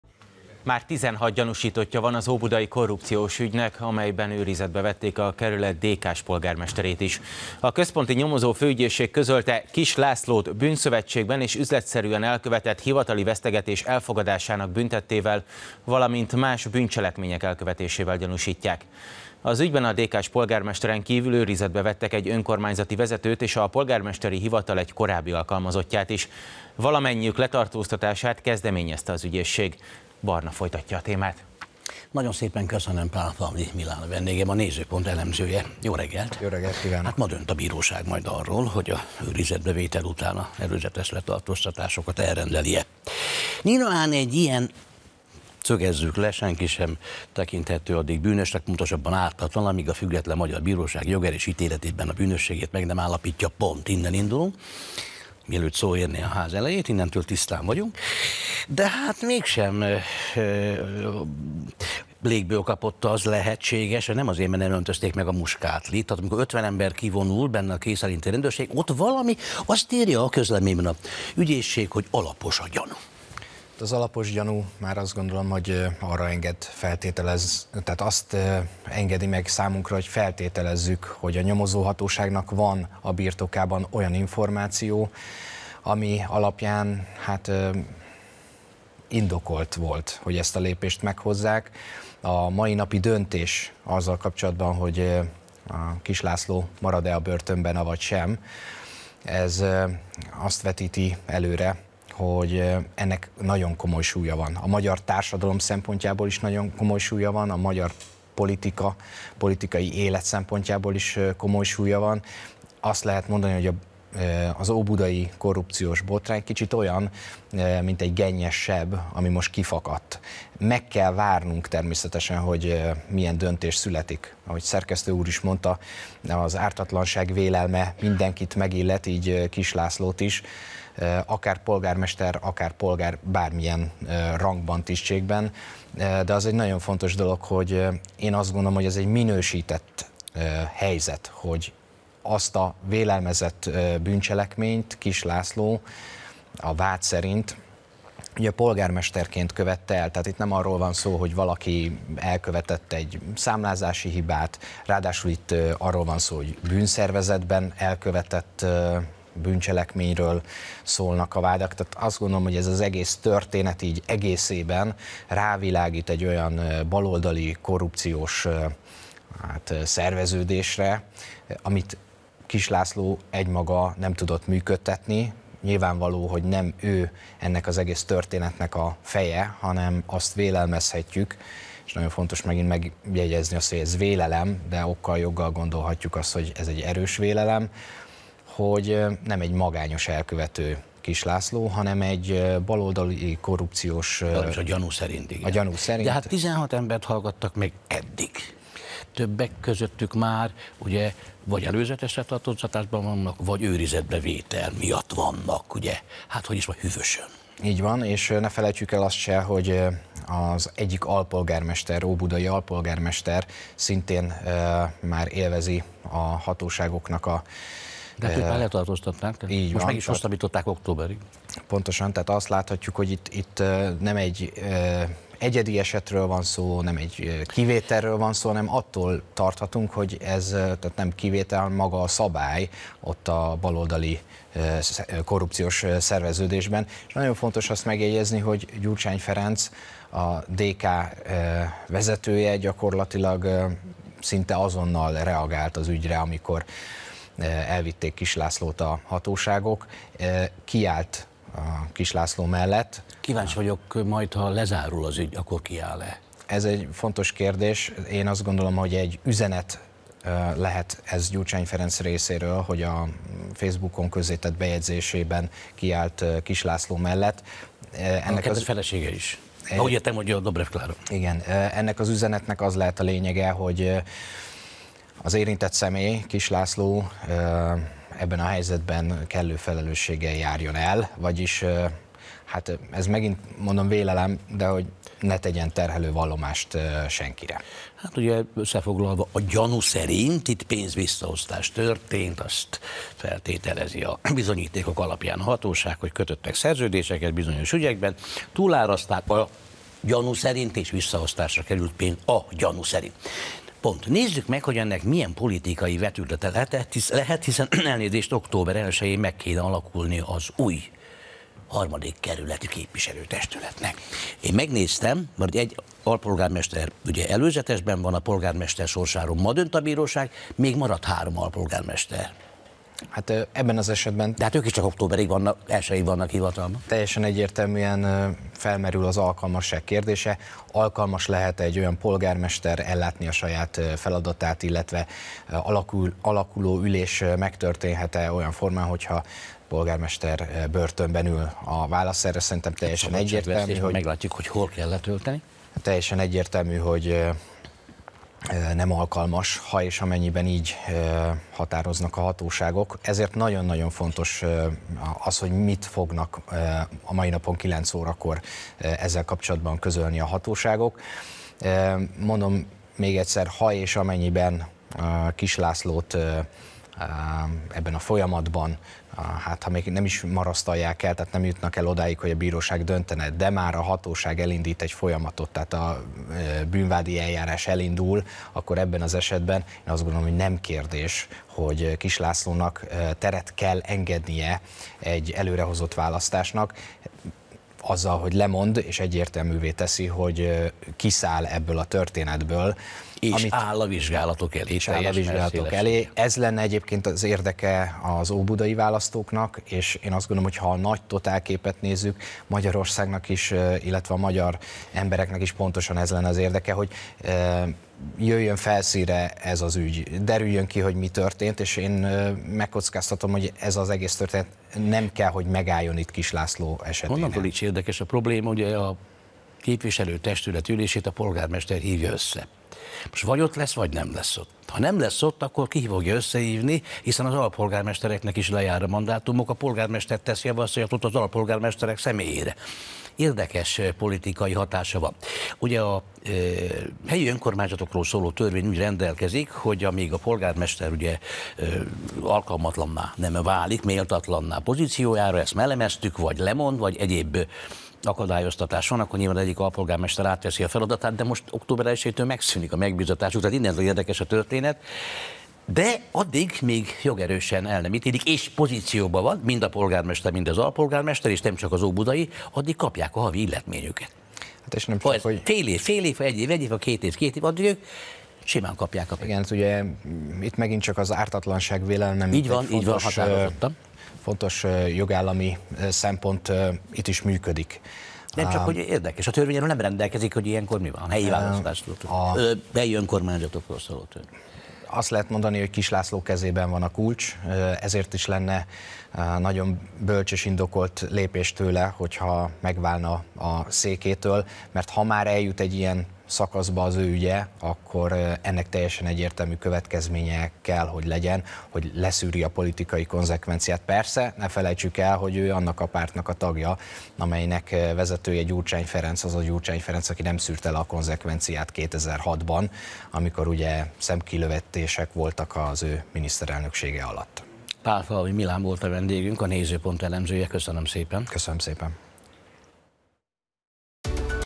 Már 16 gyanúsítottja van az óbudai korrupciós botránynak, őrizetben van a DK-s polgármester
Már 16 gyanúsítottja van az óbudai korrupciós ügynek, amelyben őrizetbe vették a kerület DK-s polgármesterét is. A Központi Nyomozó Főügyészség közölte: Kiss Lászlót bűnszövetségben és üzletszerűen elkövetett hivatali vesztegetés elfogadásának bűntettével, valamint más bűncselekmények elkövetésével gyanúsítják. Az ügyben a DK-s polgármesteren kívül őrizetbe vettek egy önkormányzati vezetőt és a polgármesteri hivatal egy korábbi alkalmazottját is. Valamennyiük letartóztatását kezdeményezte az ügyészség.